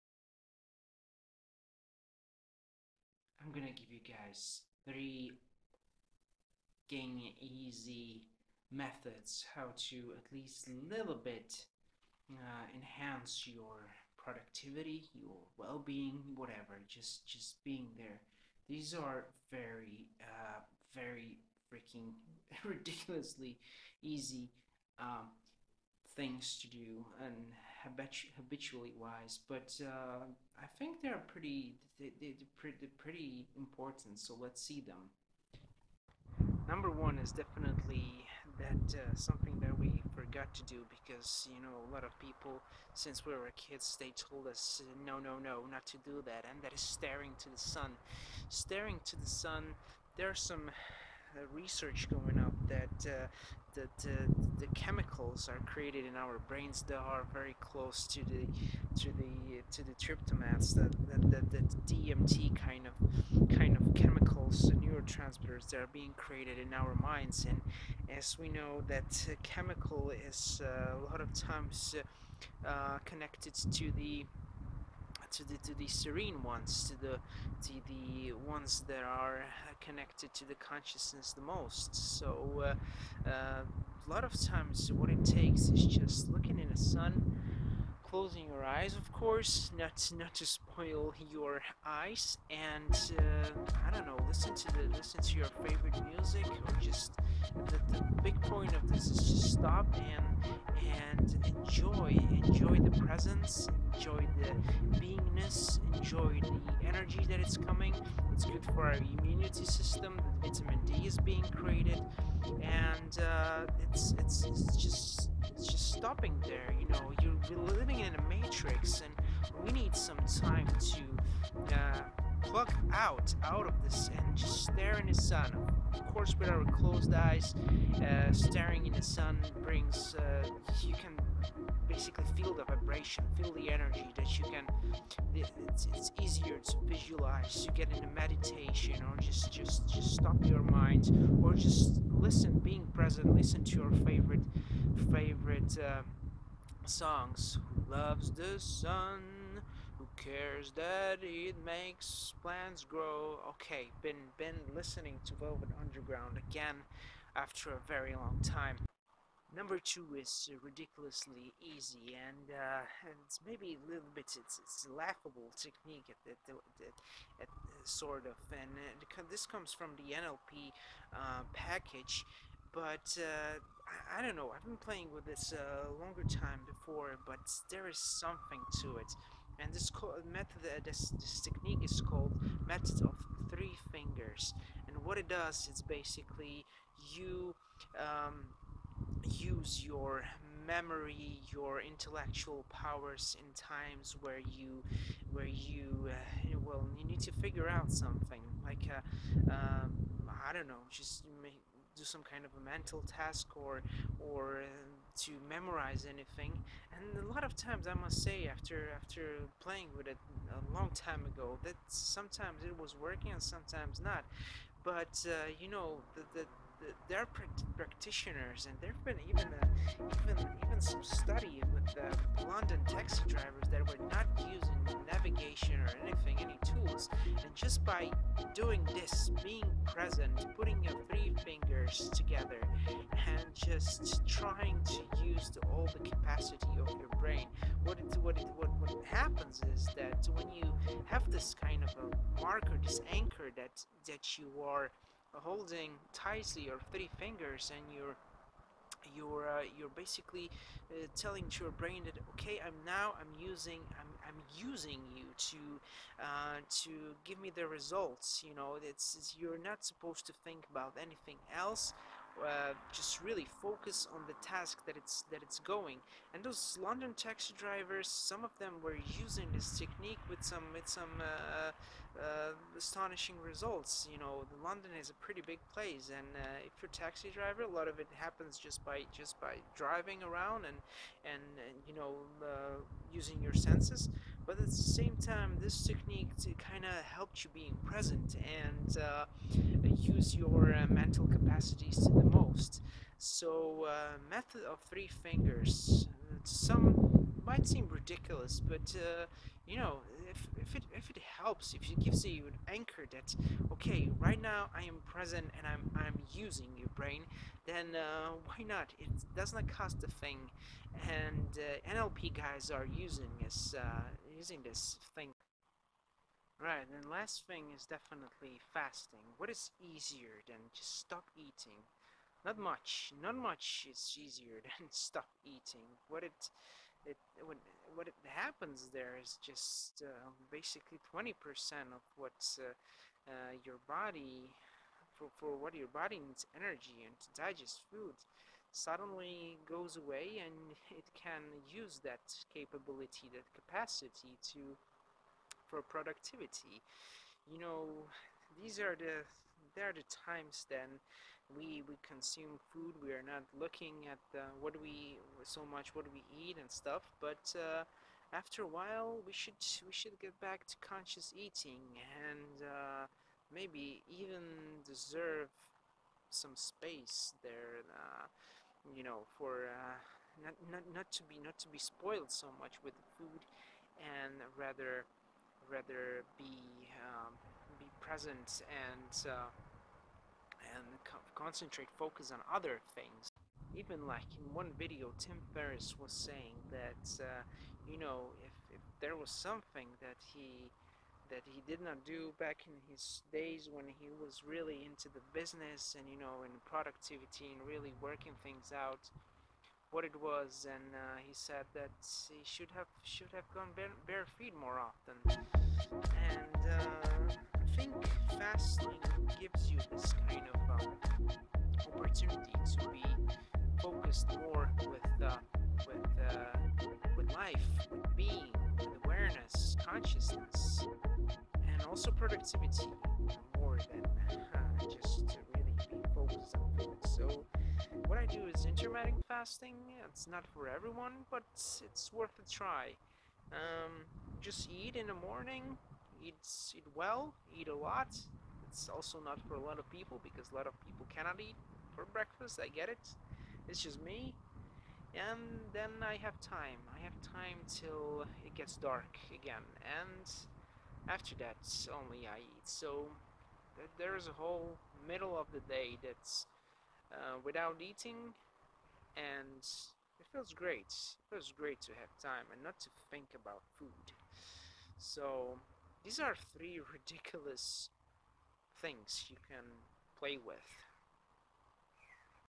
3 Ridiculously easy Techniques to improve you
3 Ridiculously easy Techniques to improve you

Here are 3 ridiculously easy to implement things that may help with well-being and productivity.

Well, intermittent fasting is not so easy for everyone.

If it’s implemented and practiced it may be a major booster.

Because you free your capacities from eating to focus more on yourself.

Whether these are well - being (consciousness) techniques or productivity  - work.

The first one is tremendously important.

Even though it is almost ridiculous.

But, yeah it has to be mentioned because matrix people say we should not. And it can be a
a major source of inspiration, energy in our lives.

stop copying